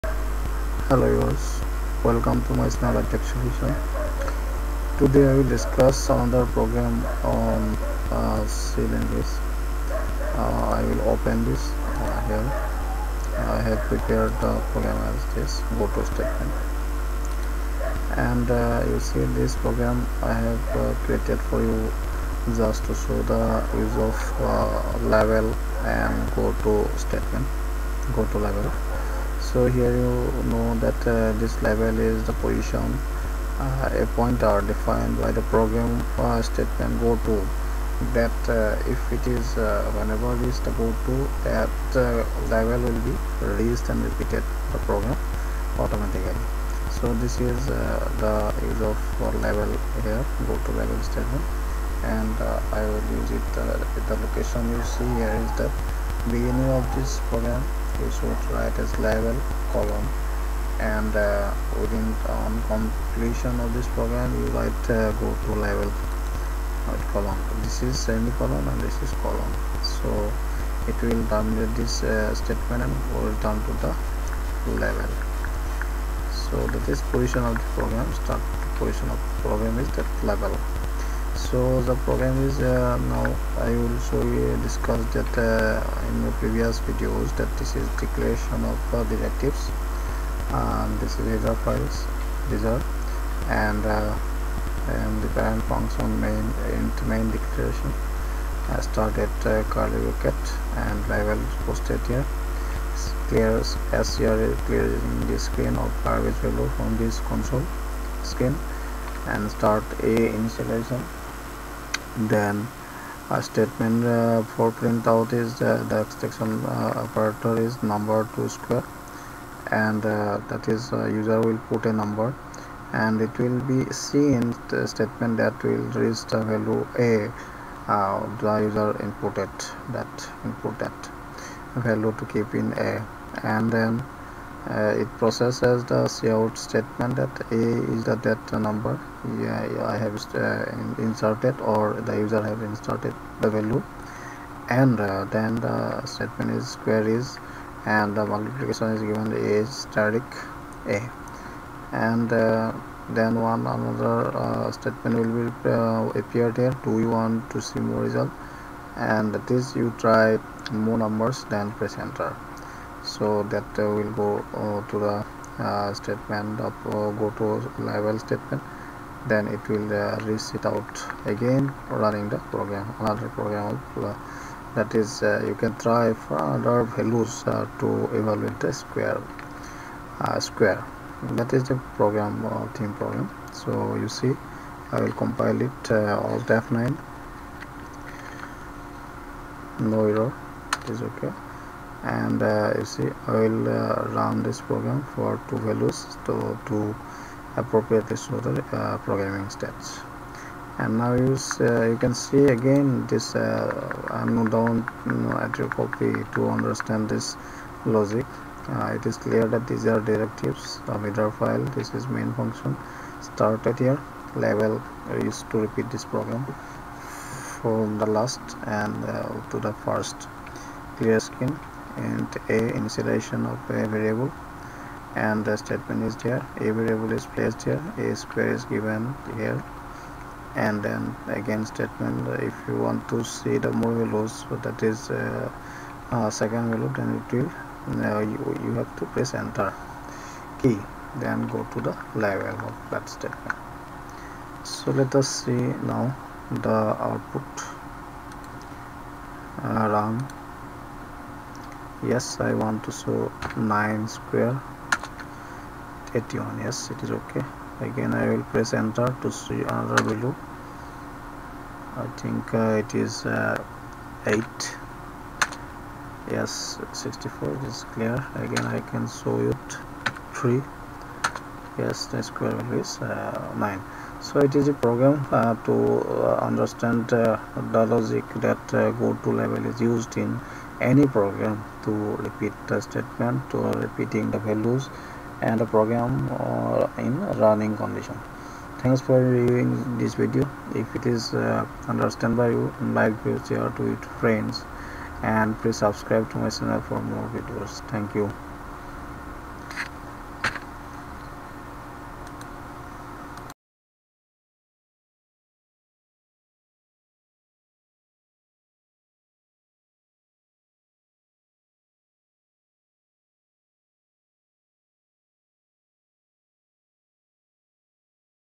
C PROGRAM USING GOTO LABEL
C PROGRAM USING GOTO LABEL, how to set label to repeat a program initialization without exiting from the main program; see the practical also to find  square of any number by itechsolution